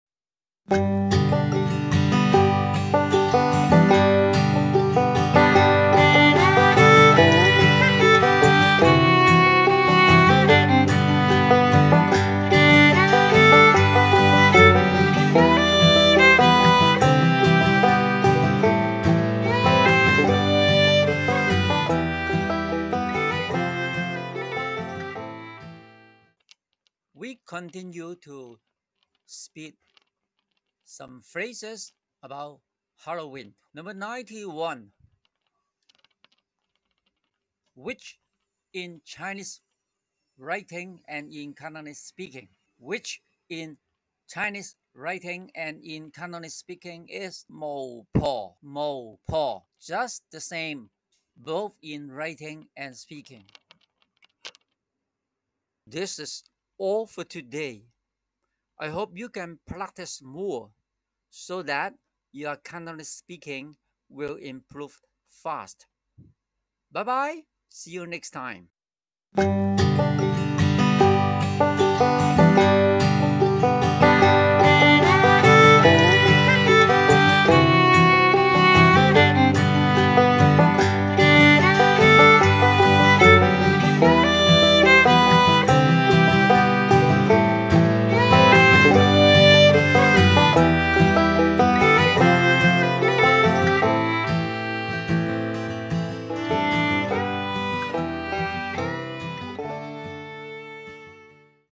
【Cantonese Speaking 廣東話點講】#32 No. 91😡 Witch How to speak witch in Cantonese
To-day's content :
#32 No.91.  😡 Witch
 -Witch in Chinese writing and in Cantonese speaking.
   Witch in Chinese writing and in Cantonese speaking is
  巫婆 ( Just the same both in writing and speaking

Thank-you for watching :
#32 No 90 Pumpkin 南瓜 Halloween 萬聖節 哈囉喂